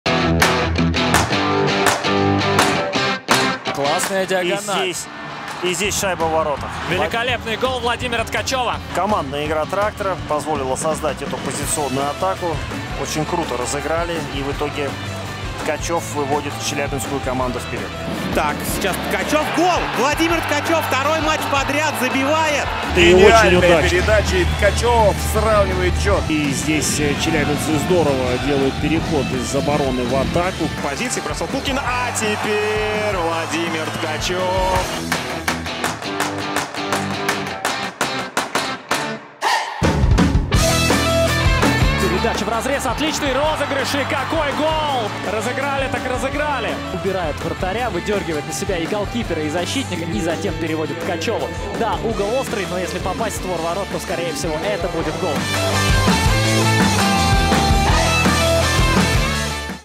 Vladimir Tkachyov, Traktor. Going to Beijing 2022
The fifth scorer among the forwards of Traktor Vladimir Tkachev (9+20 in 48 matches) returned to the Russian national team and for the first time in his career got an invitation to Olympics.
Пятый бомбардир среди нападающих «Трактора» Владимир Ткачёв (9+20 в 48 матчах) вернулся в сборную России и впервые в карьере попал в заявку команды на Олимпийские игры. Умный центр, неплохо играющий на точке вбрасывания (53,5 % в сезоне), точно пригодится российской команде.